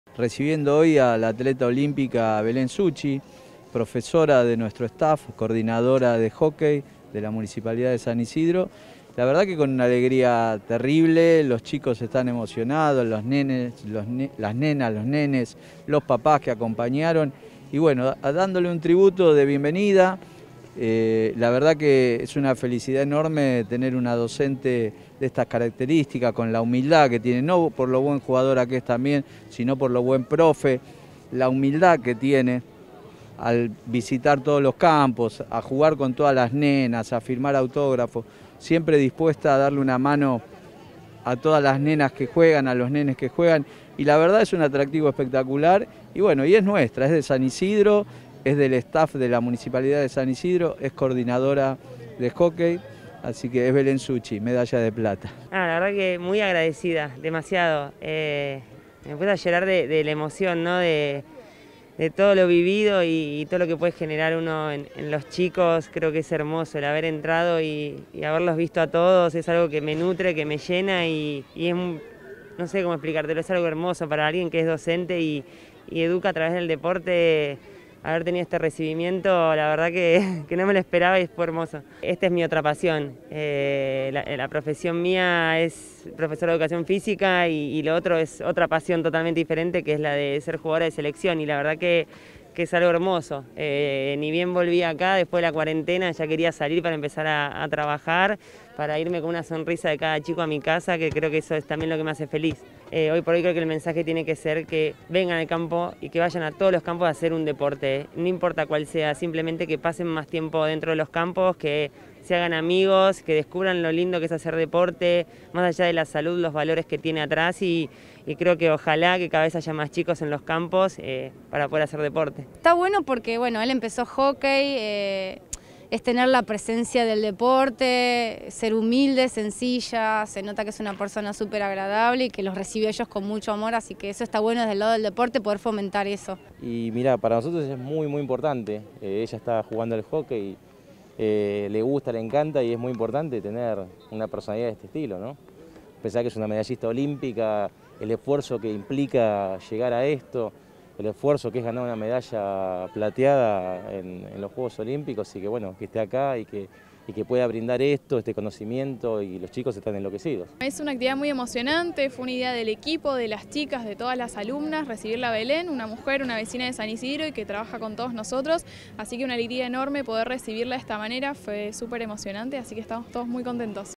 Emocionante recibimiento de la medallista olímpica Belén Succi en San Isidro
Bajo una lluvia de aplausos y ovaciones, sus alumnos, los padres, compañeros y autoridades municipales le dieron la bienvenida a la Leona, que da clases de hockey en los campos de deportes de San Isidro.

Bajo una lluvia de aplausos y ovaciones, la deportista Belén Succi -que obtuvo la medalla de plata en los últimos Juegos Olímpicos de Tokio- tuvo un emocionante recibimiento en el Campo de Deportes Nº 4 de Martínez. La Leona enseña hockey a chicos y chicas de distintas edades en los campos de deportes municipales de San Isidro.

¨Es hermoso lo que estoy viviendo. Estoy muy agradecida. Para alguien que además es docente, ver el recibimiento de los chicos, de los padres y de todos, me llena de orgullo¨, afirmó Succi.

A su lado, José María Goyanes, director general de Deportes de San Isidro, destacó: "Es una alegría enorme tener a una atleta como ella trabajando con nosotros. Tiene una humildad única, juega con los chicos en los campos y les enseña los valores del deporte. Son importantes estos tipos de recibimiento, destacar a nuestros deportistas olímpicos y agradecerles por representarnos".

También participaron la directora pedagógica Martha Chitti, los jefes de campos, regentes, profesores, padres, alumnos y alumnas.

La coordinadora municipal, Macarena Posse, resaltó “el compromiso de Belén con los chicos,  la importancia de hacer deporte y que los alumnos puedan tener a una referente como Belén en las clases”.

La arquera de la selección mayor de Las Leonas, entre sus palmarés tiene un campeonato del mundo, una medalla de bronce en los Juegos Olímpicos de Pekín y seis Champions Trophy.

"Es emocionante ver como Belén trabaja con nuestros hijos. Hace tiempo que mi hija viene al campo y cada vez que vuelve a casa se encuentra feliz y emocionada. Hay que valorar el trabajo que hacen los profesores y el municipio", concluyó Graciela Martín, mamá y vecina de Martinez.

Los interesados en sumarse a las clases de hockey se pueden comunicar telefónicamente al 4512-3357, o ingresar